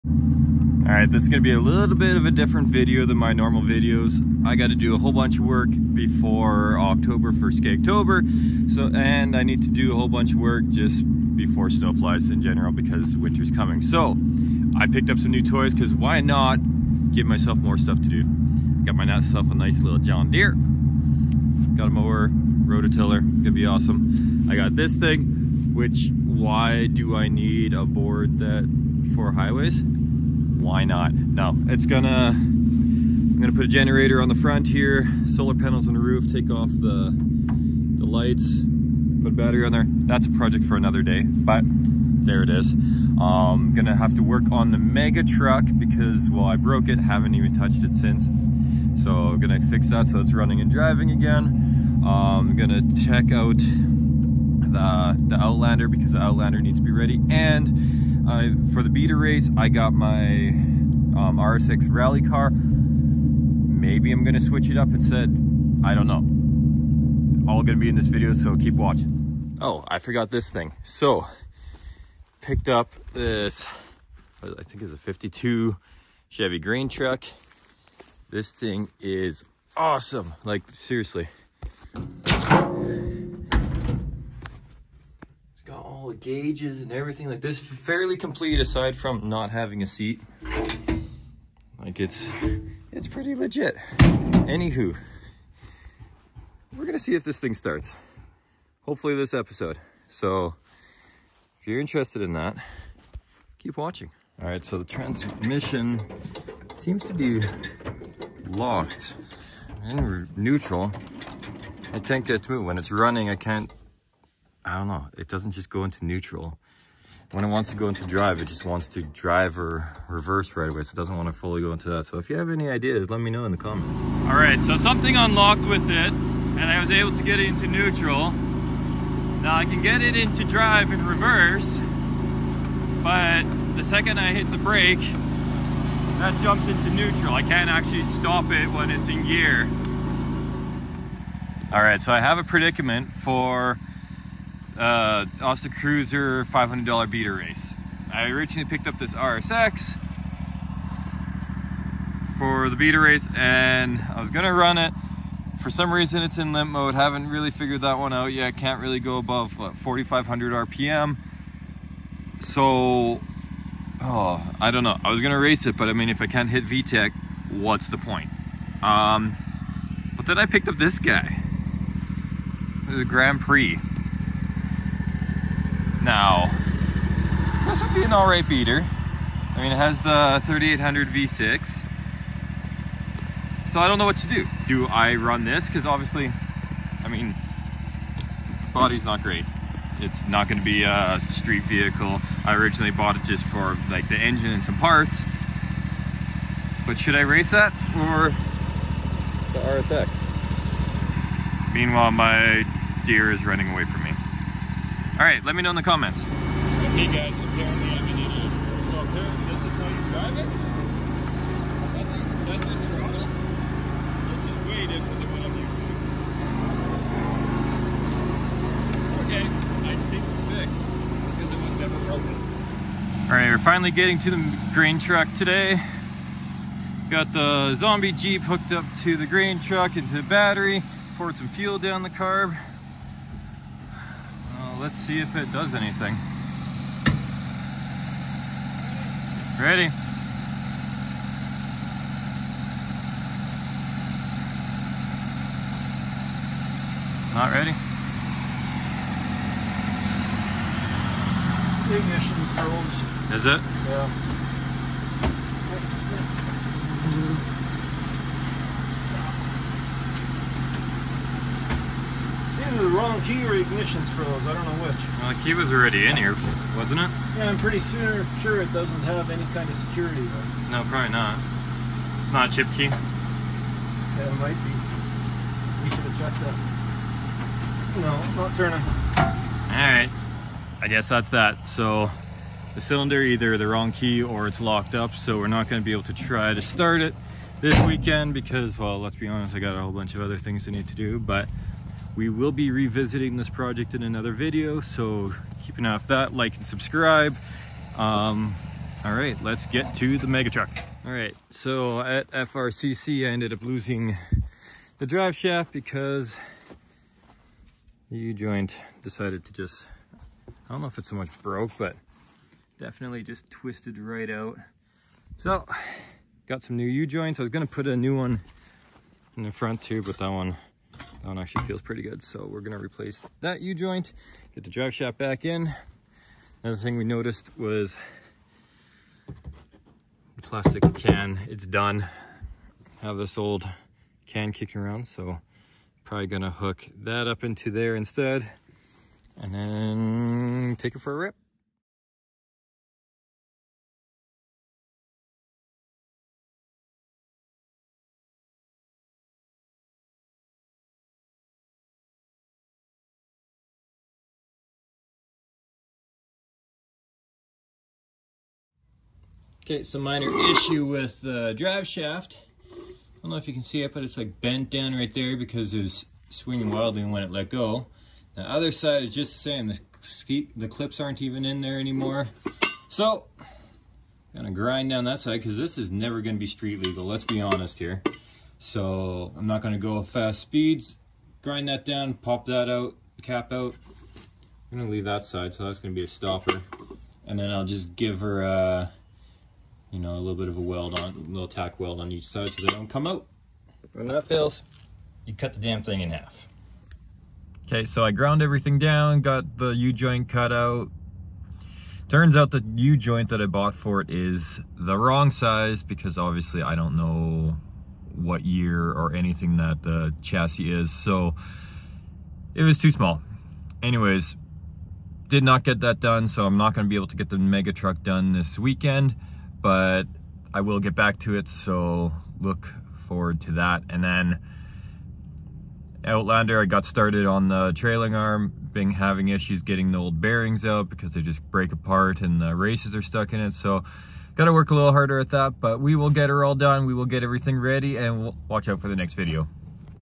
New Projects! And Fixing The Mega Truck
More work on projects, and lots more to go!
Mega truck, new tow vehicle, rally car, and the outlander.